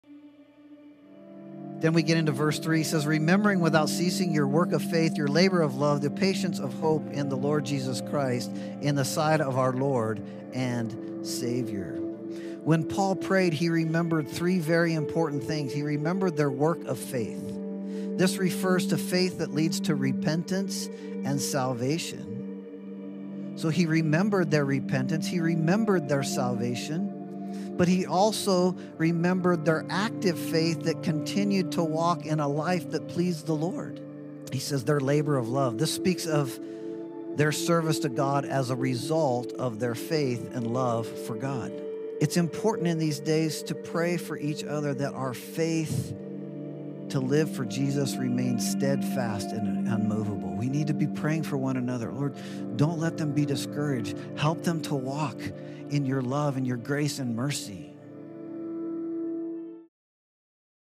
Work of Faith?
Pastor Steve linked ‘labor of love’ in first Thessalonians verse 3 to an ‘active faith’ What is active faith? What are some ways that we can live the Gospel every day?